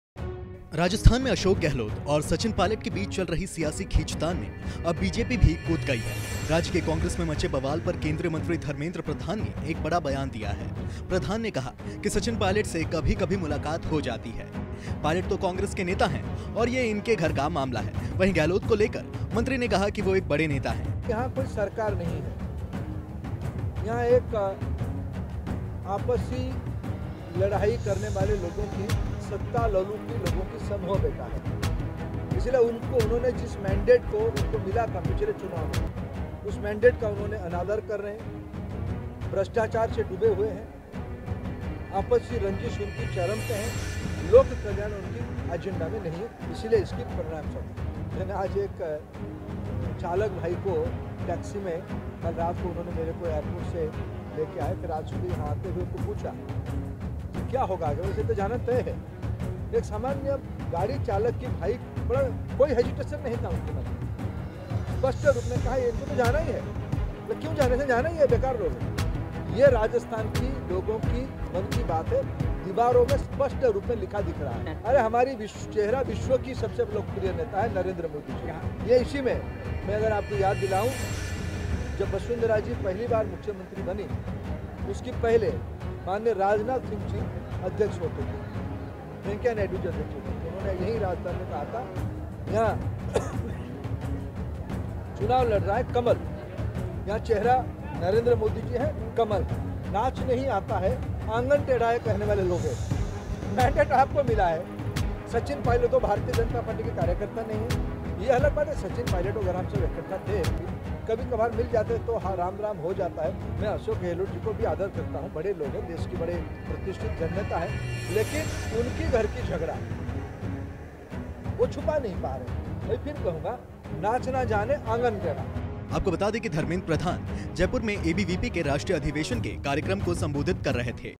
Rajasthan Politics: Ashok Gehlot और Sachin Pilot की जंग में कूदी BJP, केंद्रीय मंत्री ने कसा तंज
TODAY Live digital  NEWS with KNOWLEDGE